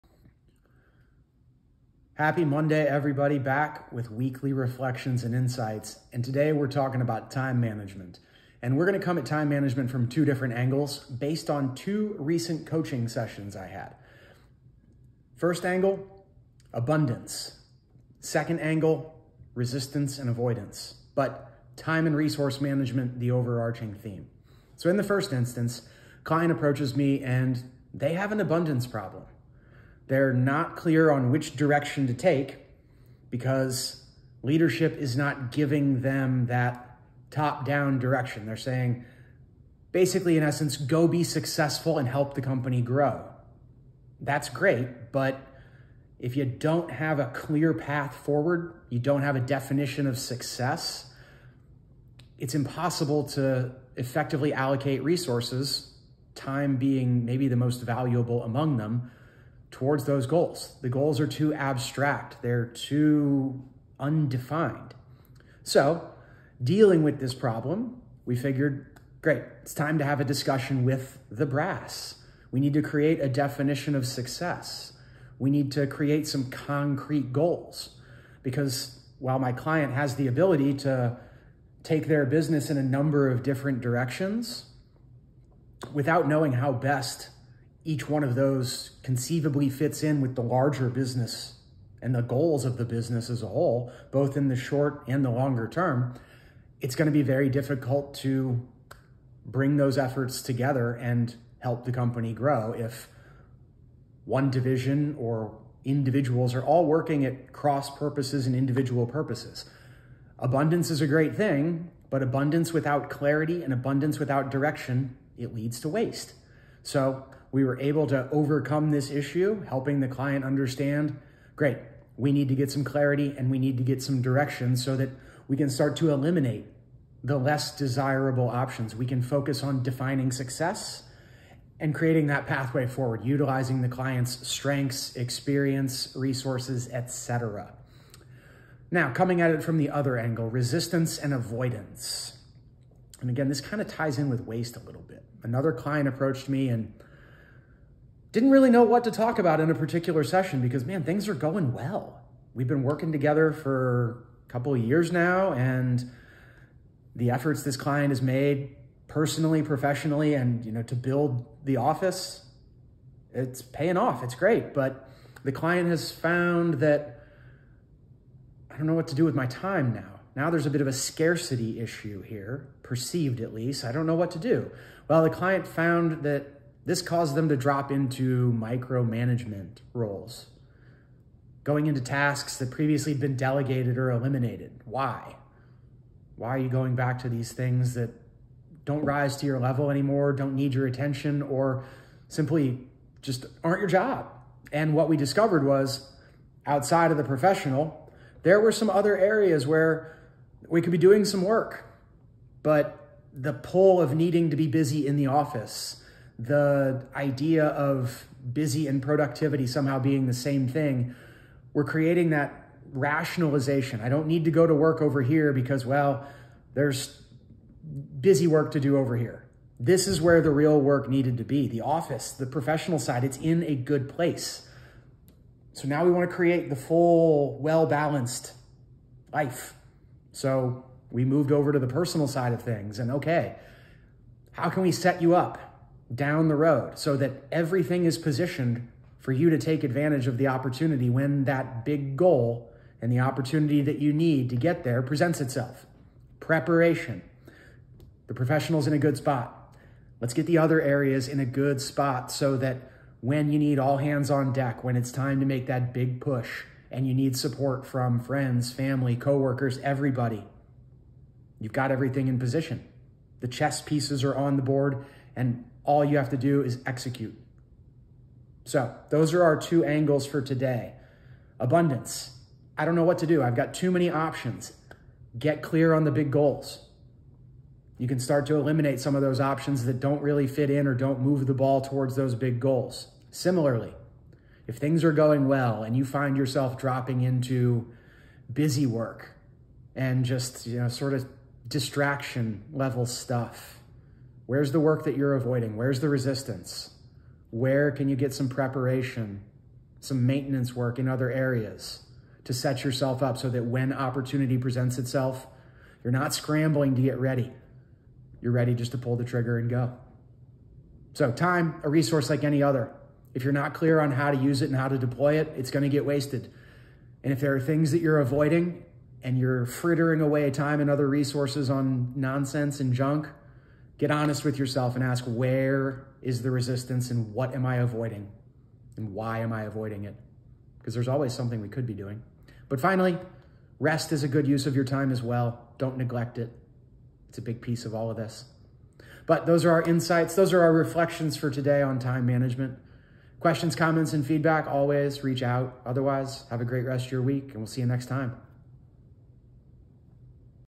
Weekly Reflections & Insights 6/5/23 - Time Management
Matt Beckmann provides insights on time management based on two recent executive coaching sessions.